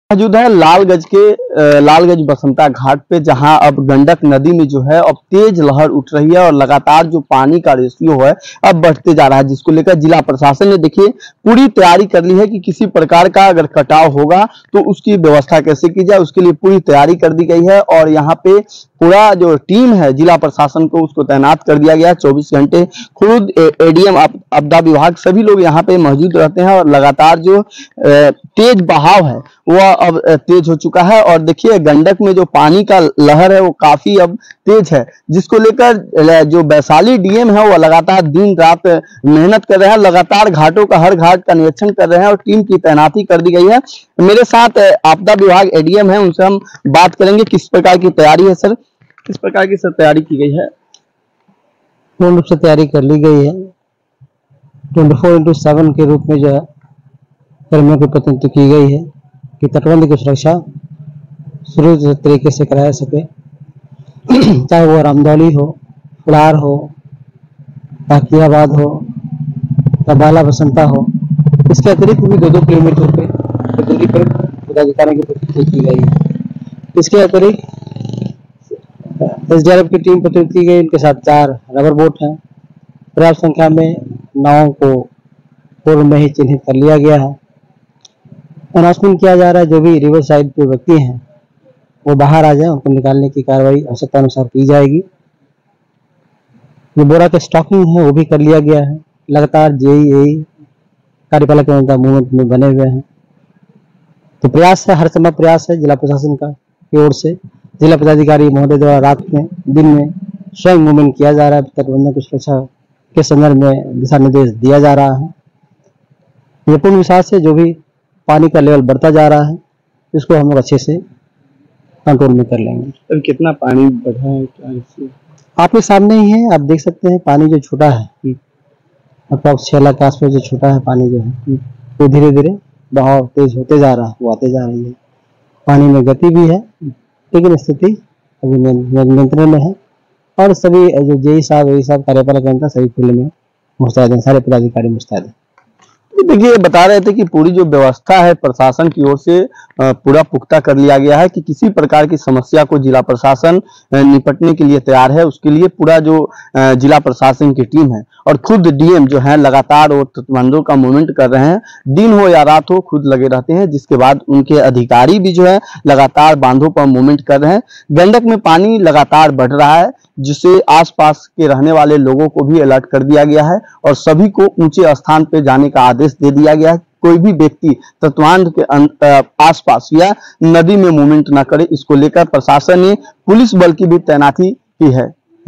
Bihar Flood News: Gandak के जलस्तर में उछाल, Vaishali में बाढ़ की चेतावनी!
Due to the continuous rise in water levels in the Gandak River, the problems faced by the residents are increasing. Seeing the rising water levels, the process of evacuating people living along the embankments has begun. The administration has alerted people living near various ghats, including Hajiapur's Kaun Hara Ghat, Kadam Ghat, Club Ghat, Bala Das Math, Lalganj, Basant Ghat, and Fulhar Ghat, through announcements via microphone to stay vigilant. Vaishali District Magistrate Yashpal Meena is on high alert with his teams 24 hours a day regarding the rising water levels in the Gandak River.
गंडक में लगातार जलस्तर बढ़ने के कारण अब लोगो की परेशानी बढ़ रही है। जलस्तर बढ़ते देख कर तटबंध किनारे रह रहे लोगो लोगों को अब हटाने की प्रक्रिया शुरू कर दी गई। हाजीपुर के कौन हारा घाट कदम घाट क्लब घाट बाला दास मठ लालगंज बसंत घाट फुलहर घाट सहित तमाम घाटों के नजदीक रह रहे लोगों को प्रशासन के द्वारा माइक के जरिए प्रचार कर अलर्ट रहने को कहा गया। गंडक में बढ़ते जलस्तर को लेकर वैशाली डीएम यशपाल मीणा 24 घंटे अपने टीमों के साथ अलर्ट मोड में है।